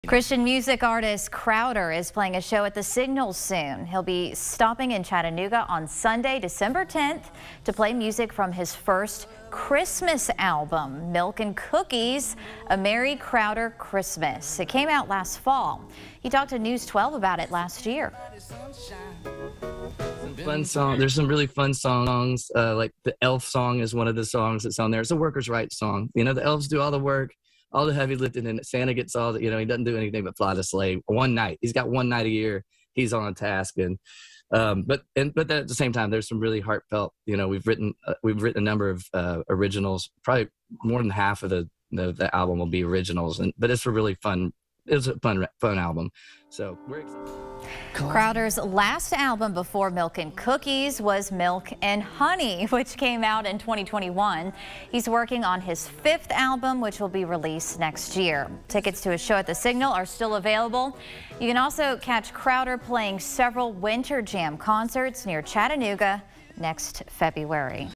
Crowder to play new Christmas album at The Signal
Christian music artist, Crowder, is playing a show at The Signal soon.